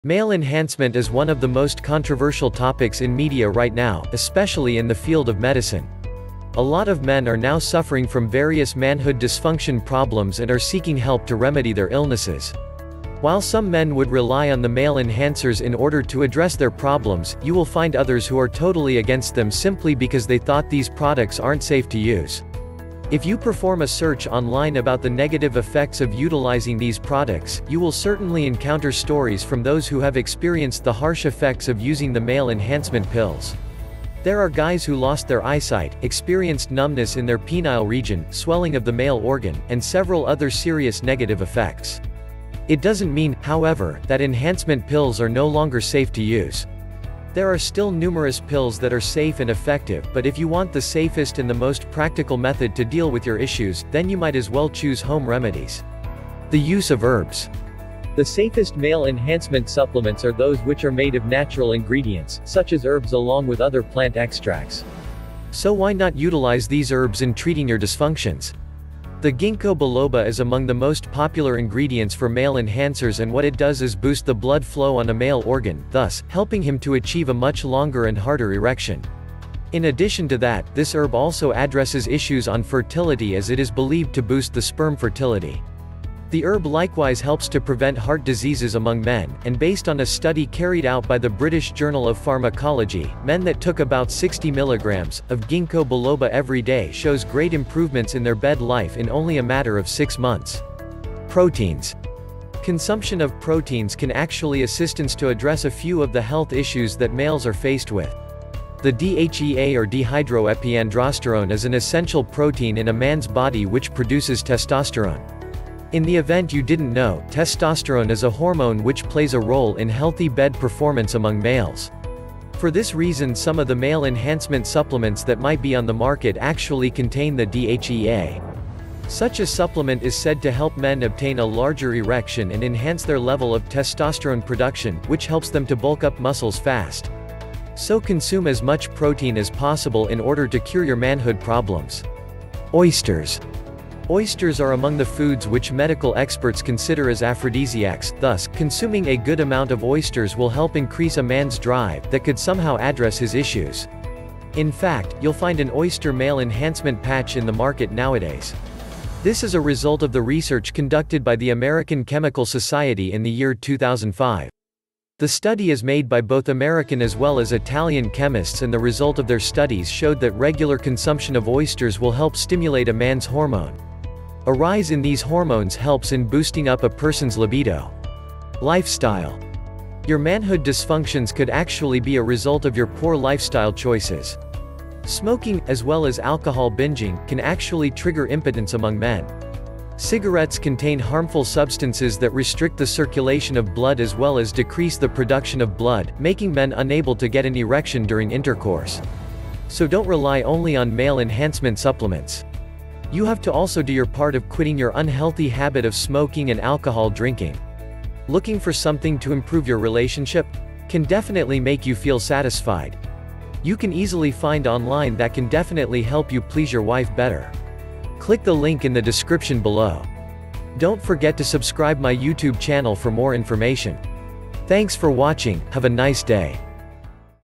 Natural Home Remedies For Male Enhancement
Male enhancement is one of the most controversial topics in media right now, especially in the field of medicine. A lot of men are now suffering from various manhood dysfunction problems and are seeking help to remedy their illnesses. While some men would rely on the male enhancers in order to address their problems, you will find others who are totally against them simply because they thought these products aren't safe to use.

If you perform a search online about the negative effects of utilizing these products, you will certainly encounter stories from those who have experienced the harsh effects of using the male enhancement pills. There are guys who lost their eyesight, experienced numbness in their penile region, swelling of the male organ, and several other serious negative effects. It doesn't mean, however, that enhancement pills are no longer safe to use. There are still numerous pills that are safe and effective, but if you want the safest and the most practical method to deal with your issues, then you might as well choose home remedies.

The Use Of Herbs

The safest male enhancement supplements are those which are made of natural ingredients, such as herbs along with other plant extracts. So why not utilize these herbs in treating your dysfunctions?

The Ginkgo Biloba is among the most popular ingredients for male enhancers and what it does is boost the blood flow on a male organ, thus, helping him to achieve a much longer and harder erection. In addition to that, this herb also addresses issues on fertility as it is believed to boost the sperm fertility. The herb likewise helps to prevent heart diseases among men, and based on a study carried out by the British Journal of Pharmacology, men that took about 60 mg. of Ginkgo Biloba every day shows great improvements in their bed life in only a matter of six months.

Proteins

Consumption of proteins can actually assistance to address a few of the health issues that males are faced with. The DHEA or Dehydroepiandrosterone is an essential protein in a man's body which produces testosterone. In the event you didn't know, testosterone is a hormone which plays a role in healthy bed performance among males.

For this reason some of the male enhancement supplements that might be on the market actually contain the DHEA. Such a supplement is said to help men obtain a larger erection and enhance their level of testosterone production, which helps them to bulk up muscles fast. So consume as much protein as possible in order to cure your manhood problems.